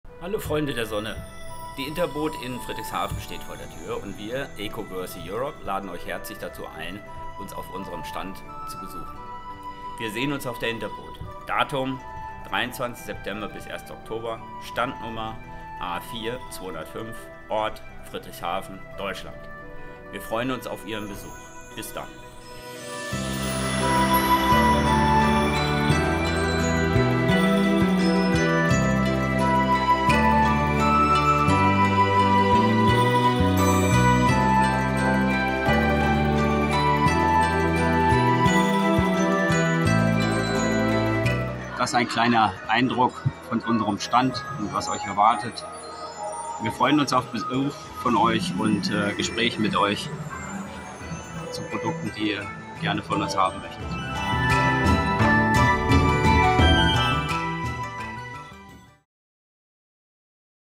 ECO-WORTHY auf der Interboot 2023
Hallo Freunde der Sonne! Die Interboot in Friedrichshafen steht vor der Tür und wir, Eco-Worthy Europe, laden euch herzlich dazu ein, uns auf unserem Stand zu besuchen. Wir sehen uns auf der Interboot. Datum 23. September bis 1. Oktober. Standnummer A4/205, Ort Friedrichshafen, Deutschland. Wir freuen uns auf Ihren Besuch. Bis dann!

eco-worthy.de